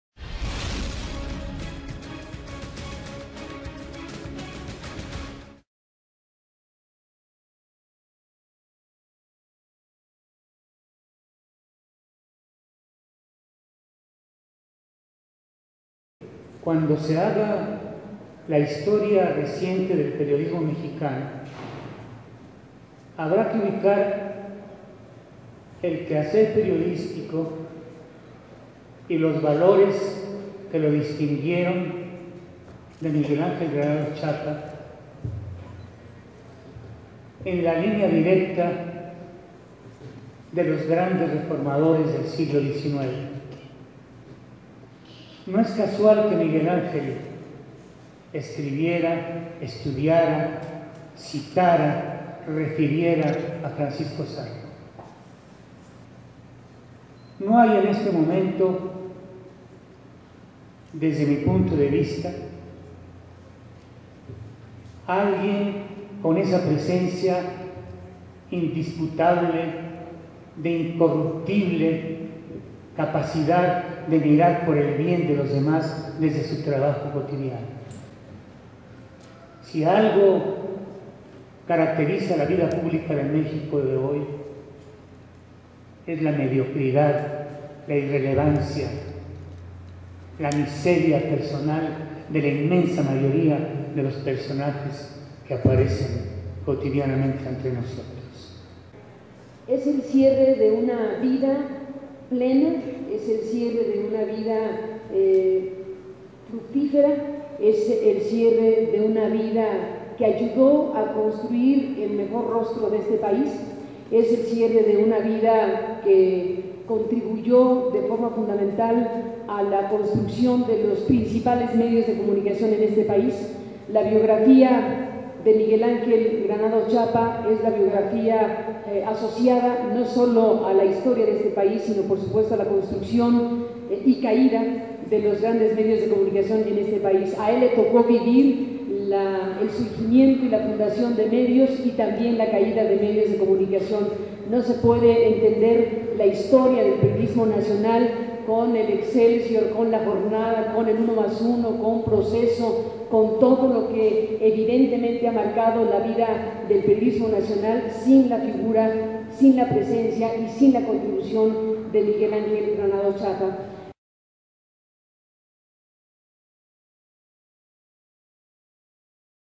Homenaje a Miguel Ángel Granados Chapa - Segunda parte
A punto de cumplirse dos años del fallecimiento de Miguel Ángel Granados Chapa compartimos algunos fragmentos del homenaje que la Asociación Mexicana de  Derecho a la Información (Amedi) le realizó al autor de la columna "Plaza Pública" en el Club de Periodistas en 2011.